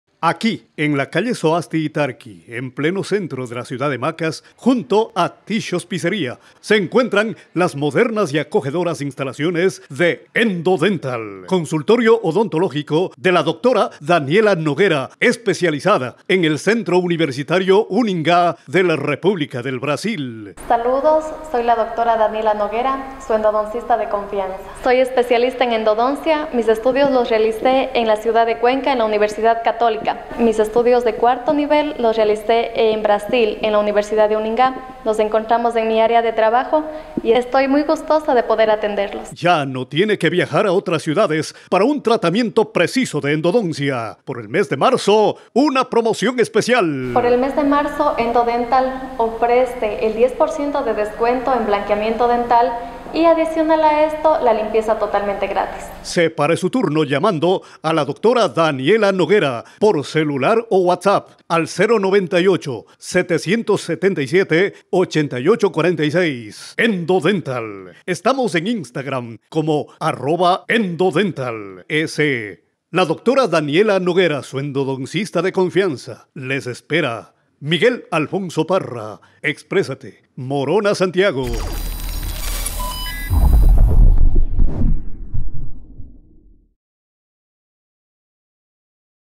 Dra. Daniela Noguera especialista en endodoncia, con estudios en Brasil, brinda servicios en Macas
INTERESANTE: Morona Santiago, ya no necesita viajar a otras ciudades del país, para un tratamiento profesional de endodoncia. En la ciudad de Macas, usted puede contar con Endo Dental - Daniela Noguera

En el centro de la ciudad de Macas se encuentran las modernas y acogedoras instalaciones de Endondental. Se trata del consultorio odontológico de la Dra. Daniela Noguera, especialista en endodoncia, con estudios realizados en Brasil.

Endo Dental - Daniela Noguera
Dirección: Soasti y Tarqui